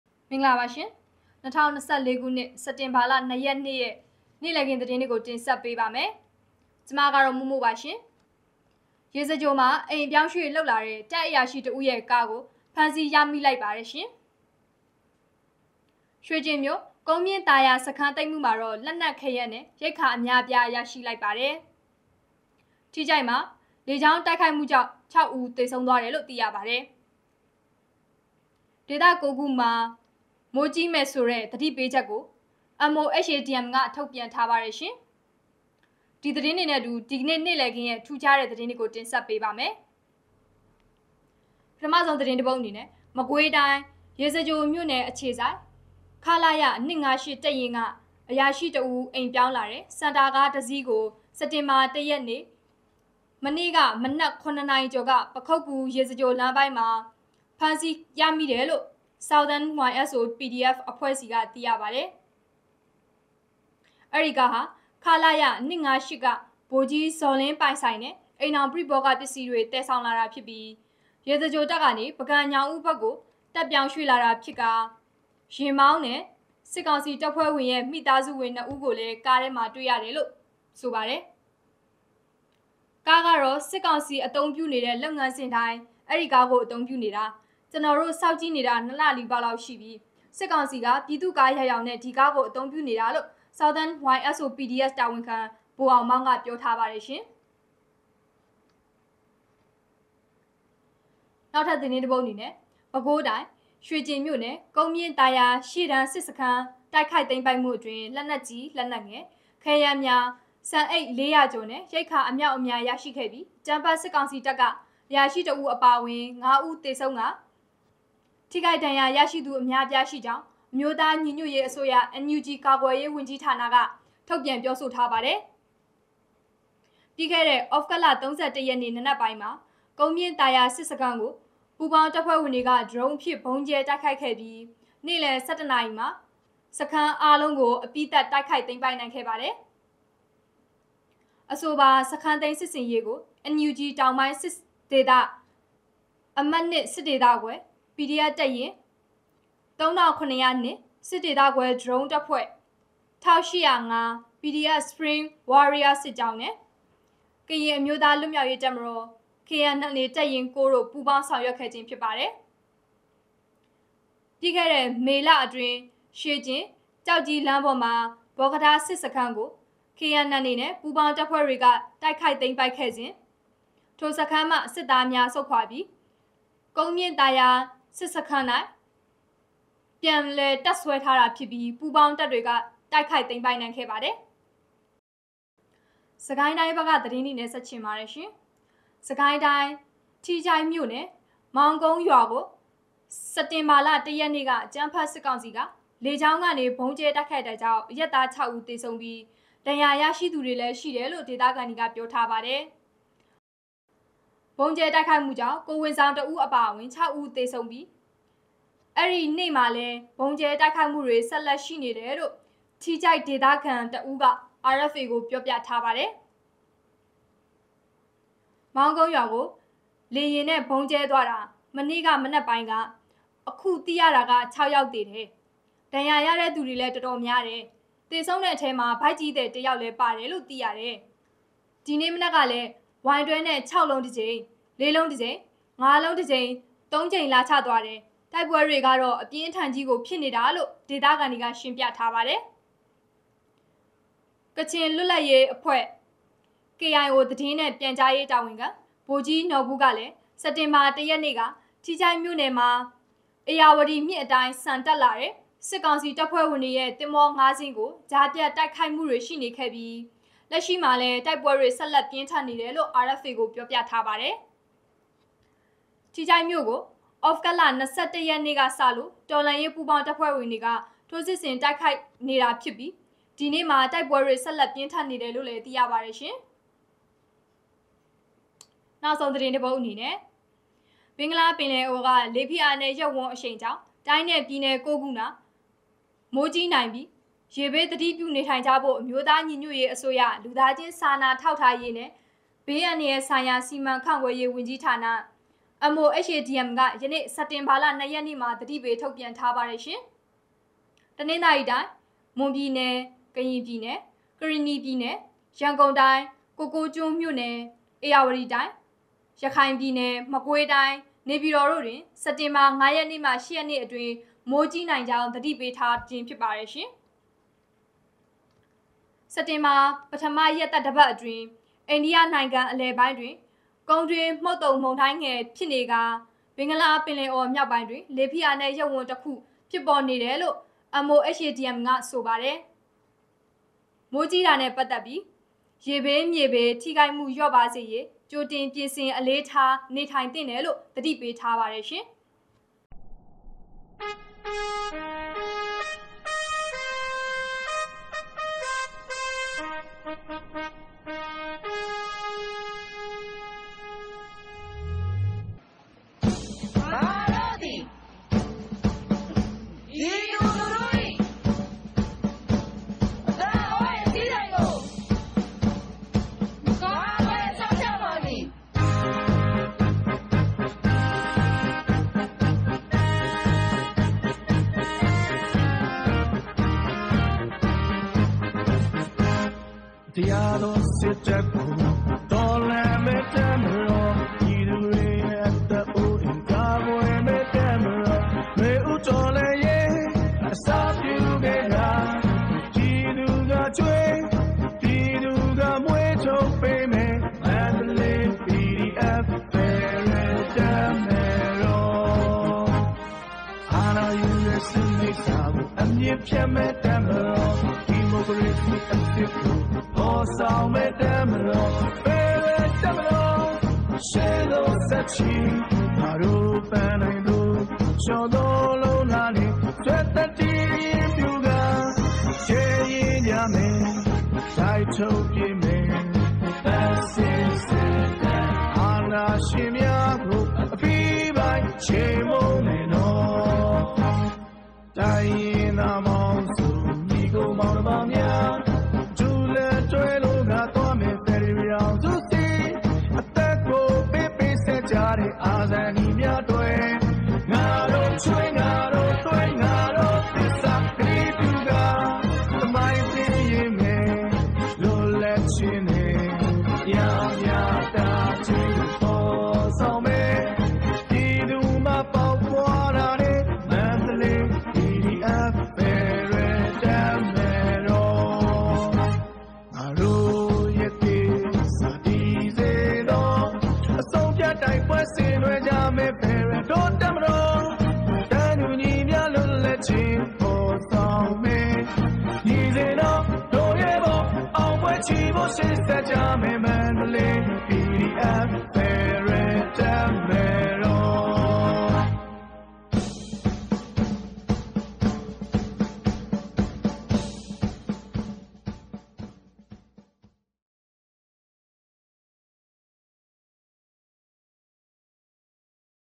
နေ့လည်ခင်းသတင်းများ - စက်တင်ဘာလ ၂ ရက်
ရေစကြိုမှာအိမ်အပြောင်းအရွှေ့လုပ်လာတဲ့ တပ်အရာရှိတဦးရဲ့ကားကို ဖမ်းဆီးရမိလိုက်ပါတယ်။
ရွှေကျင်မြို ကုန်းမြင့်သာယာစခန်းသိမ်းမှုမှာတော့ လက်နက်ခဲယမ်းနှင့် ရိက္ခာတွေအများအပြားရရှိလိုက်ပါတယ်။
ထီးချိုင့်မှာ လေကြောင်းတိုက်ခိုက်မှုကြောင့် ခြောက်ဦးသေဆုံး
ဒေသကိုးခု မိုးကြီးမယ်ဆိုတဲ့ သတိပေးချက်ကို  MOHADM ထုတ်ပြန်ထားပါတယ်။